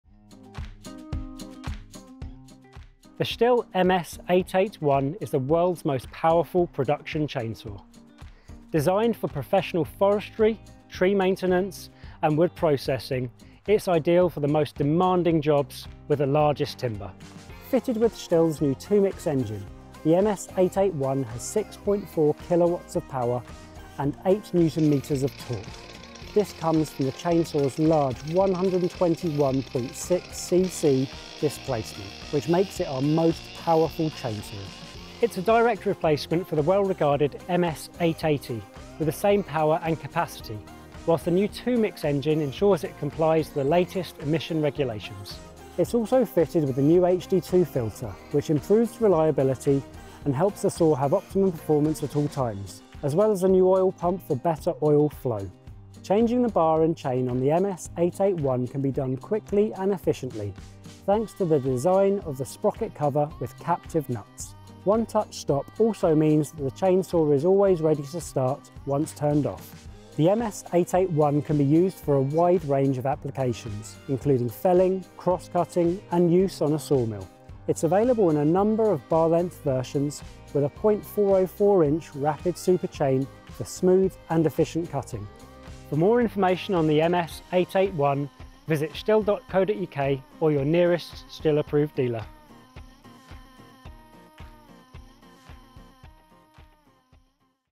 STIHL MS 881 Professional Chainsaw | Forestry Chainsaw | STIHL GB
The STIHL MS 881 is the world’s most powerful production chainsaw. This professional chainsaw has been designed for use in professional forestry, tree maintenance and wood processing and the STIHL MS 881 is ideal for the most demanding jobs with the largest timber.

It comes fitted with STIHL’s new 2-MIX engine and the MS 881 professional chainsaw has 6.4kW of power and 8.0 Nm of torque. This comes from the chainsaw’s large 121.6cc displacement which makes it our most powerful chainsaw.

It’s a direct replacement for the well-regarded MS 880 professional chainsaw, with the same power and capacity, whilst the new STIHL 2-MIX engine ensures the MS 881 complies with new EU5 emission regulations.

It’s also fitted with the new HD2 filter, which improves reliability and helps the chainsaw have optimum performance at all times, as well as a modern oil pump for better oil flow.

Changing the bar and chain on the MS 881 chainsaw can be done quickly and efficiently thanks to the design of the sprocket cover with captive nuts. One touch stop also means that the chainsaw is always ready to start once turned off.

The MS 881 can be used for a wide range of applications, including felling, crosscutting and use on a saw mill. It’s available in 30, 36 and 41” bar lengths options with a .404” Rapid Super chain for smooth and efficient cutting.

To find out more information on the MS 881 professional chainsaw, click the product link below or contact your local STIHL Approved Dealer today